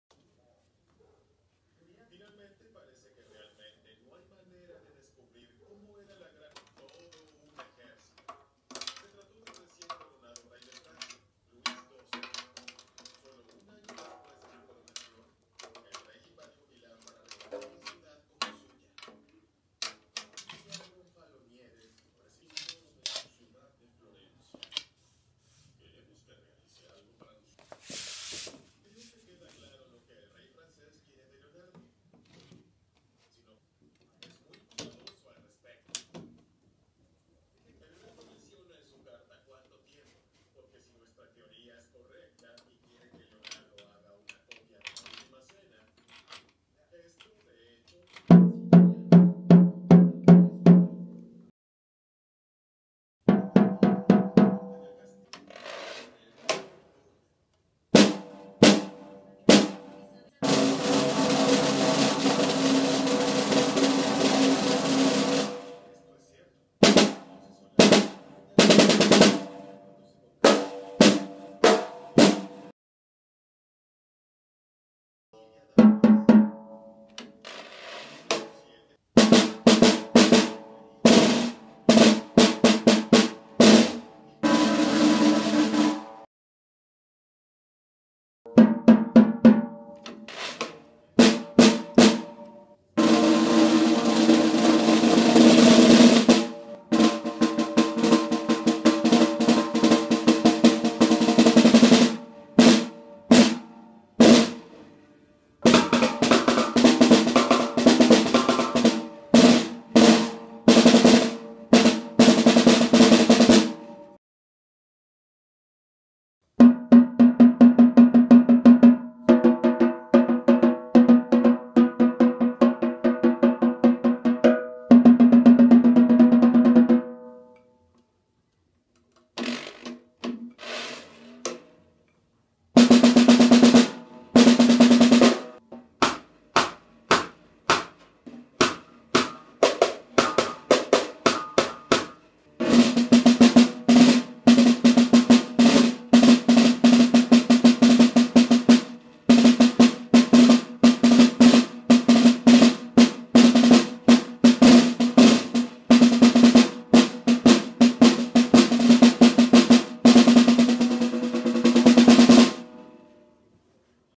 Evans HD Dry 14" (Review)
Reseña del parche Evans HD Dry, control de armónicos mediante orificios, superficie rugosa, excelente control al afinar, buen tono, Su sonidos es mas seco con un sonido profundo. Rate: 8.5/10